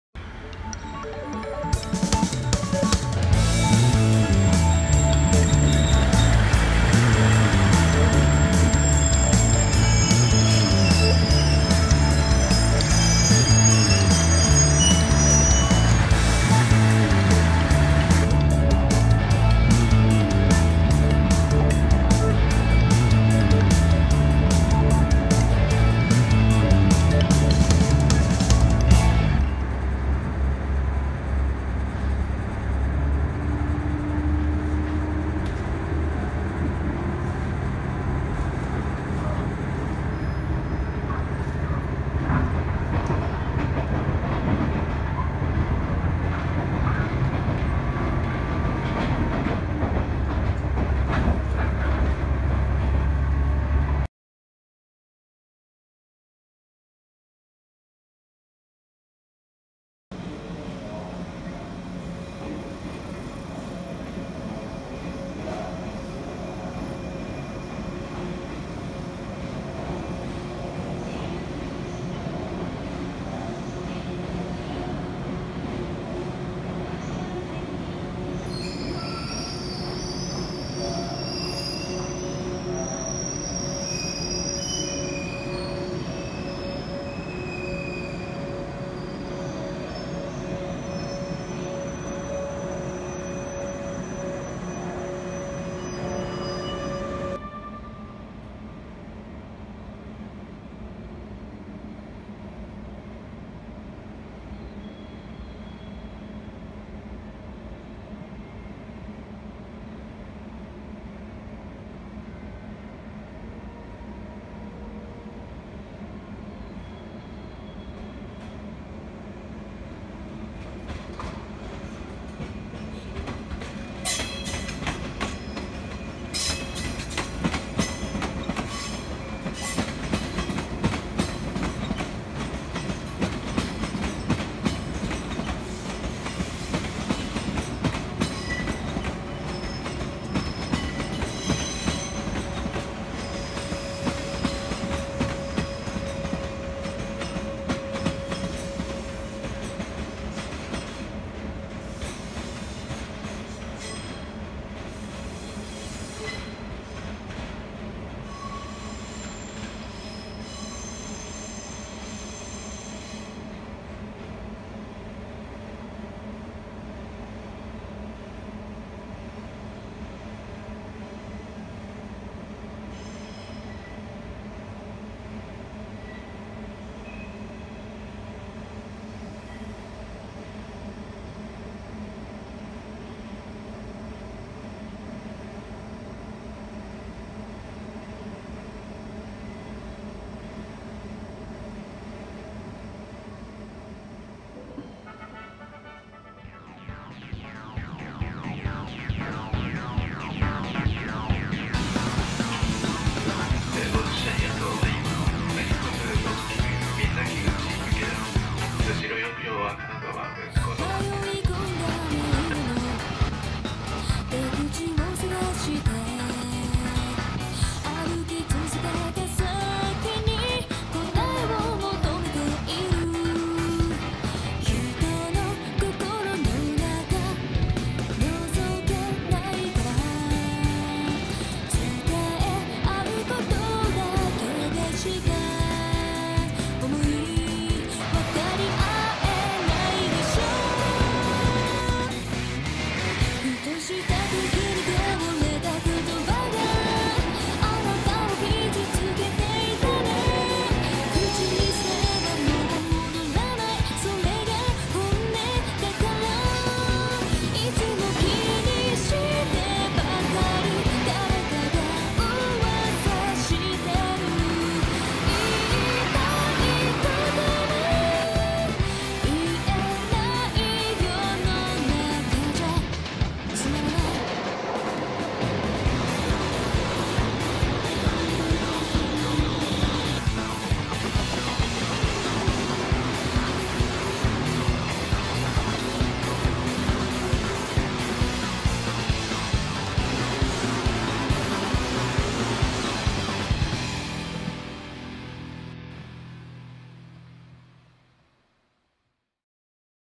【回9125M撮影!】485系列の生き残りJT"華"。Shooting the group limited express train "Hana" at Sendai station.
仙台駅にて回9125Mの撮影に臨みました。到着時刻や、入線する場所も分からないまま、私の軽く淡い憶測、希望的観測を込めての撮影でした。
At Sendai station, I tried shooting the group train "Hana" of the 485 series train.